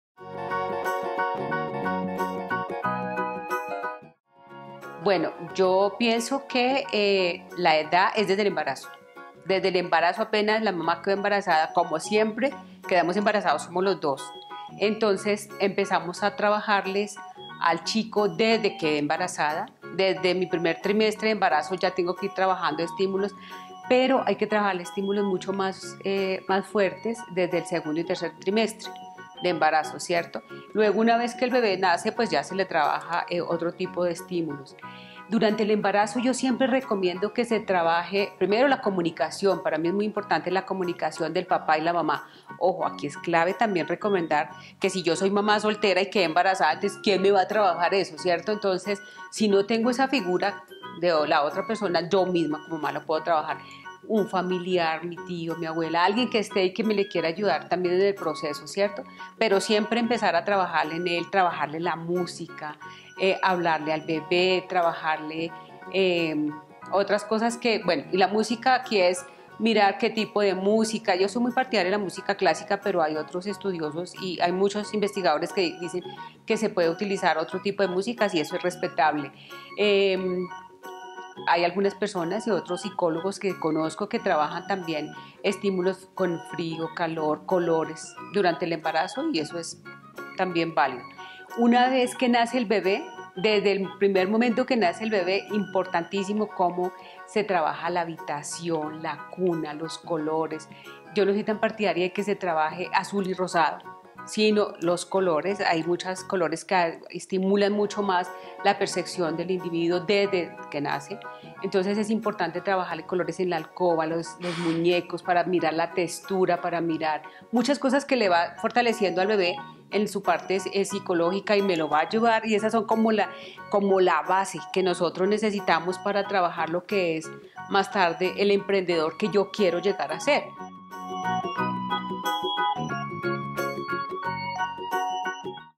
¿Desde cuando puedo estimular la mente de mi hijo?
Una de las preguntas más frecuentes es desde cuándo podemos hacer estimulación neuronal a nuestro bebé; la maestra Norma Velásques, sicóloga especialista en emprendimiento, nos explica desde qué momento y cuáles estímulos son los mejores para nuestro hijo o hija.